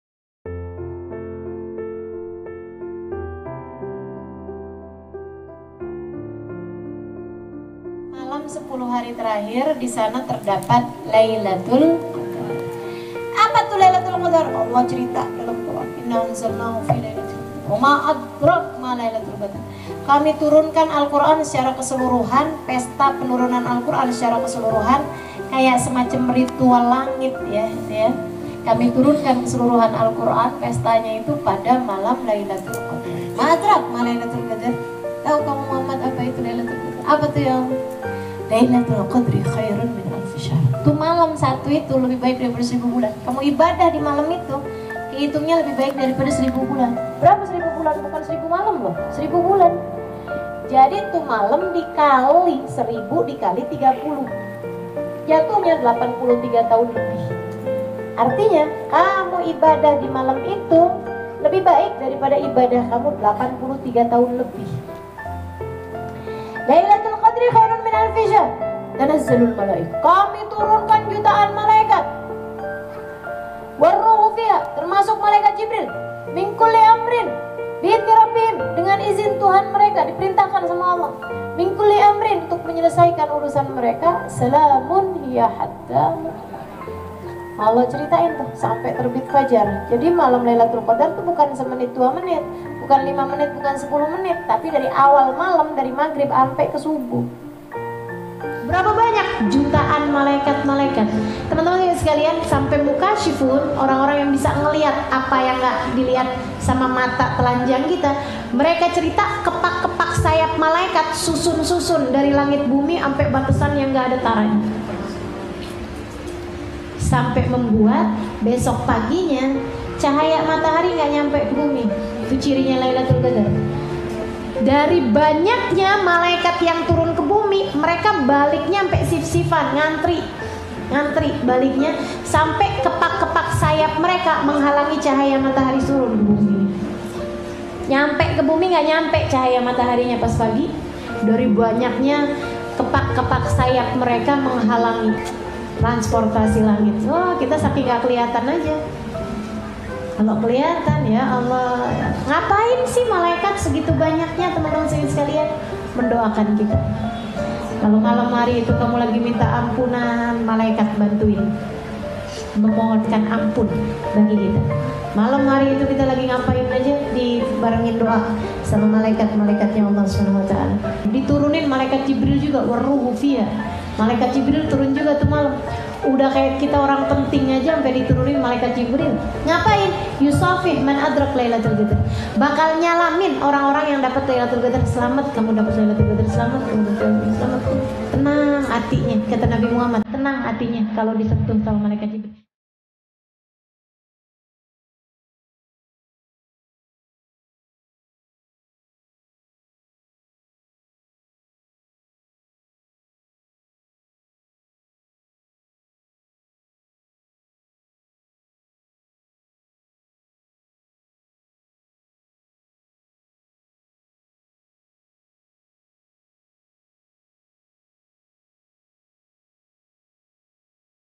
Ustadzah Halimah Alaydrus - Malam lailatul qodar
Memasuki 10 malam terakhir Ramadhan. Persiapan yuuuuk.. Agar keberkahan lailatulqodar tak terlewat dari kita.